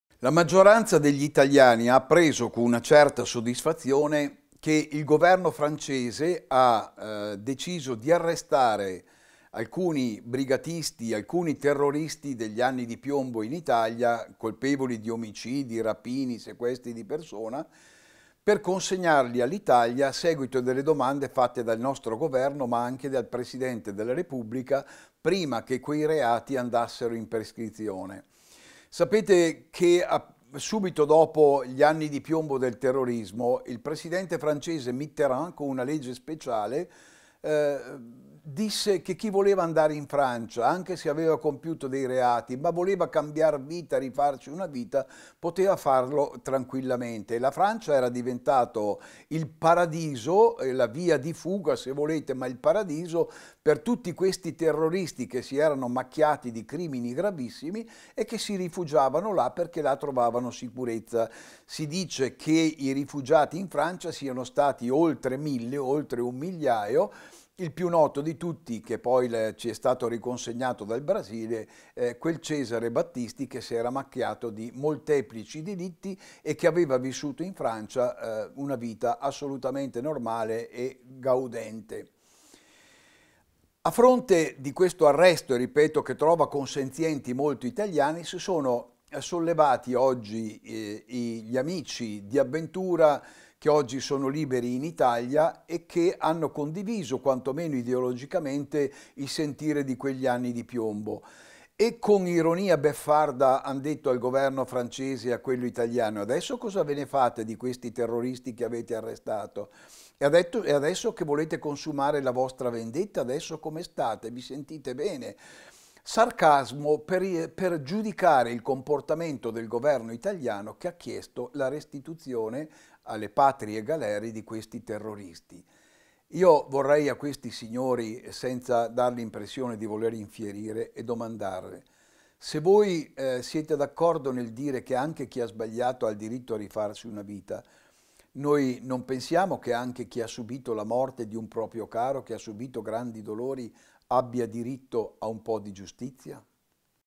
Don Bruno Fasani - Terrorismo: una pagina da chiudere
Editoriale
Don Bruno Fasani

Dopo gli arresti di 7 terroristi italiani in Francia grazie all'appoggio del presidente Macron e alla consegna di un ottavo terrorista dei tempi degli anni di piombo, ascoltiamo  la riflessione di Don Bruno Fasani sulla chiusura di una pagina di storia che sta facendo discutere molti.